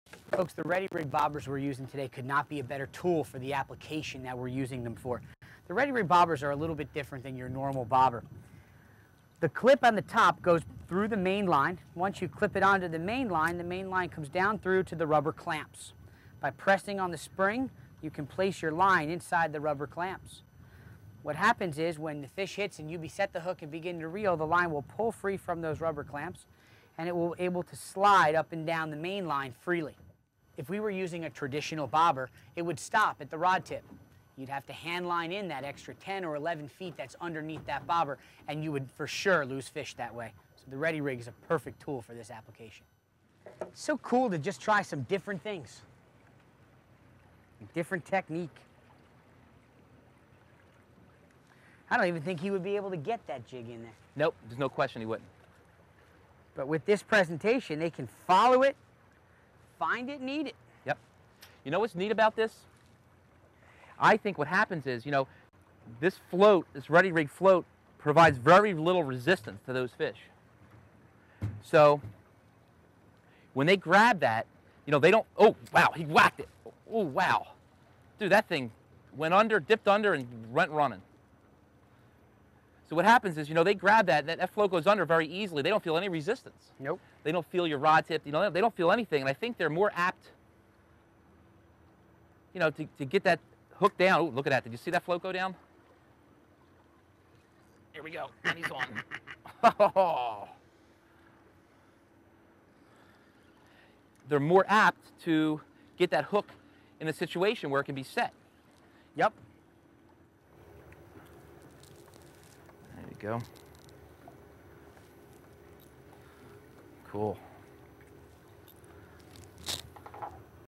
FlounderRediRig.mov
New Jersey Anglers with new technique for flounder fishing with RedIRIg